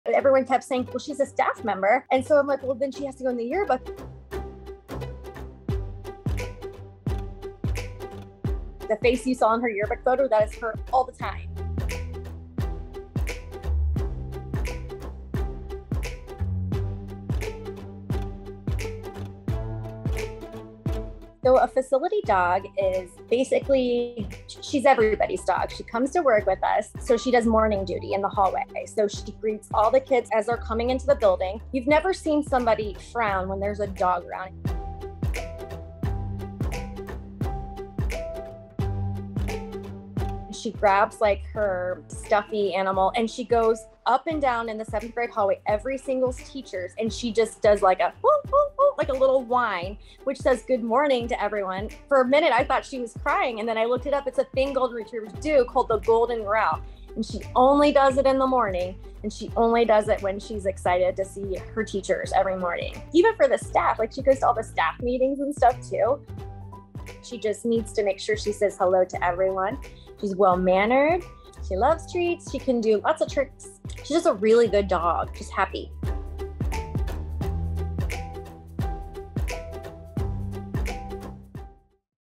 Middle school facility dog gets her own adorable yearbook photo
Meg greets the students and teachers at Goshen Middle School every morning and is even considered a staff member.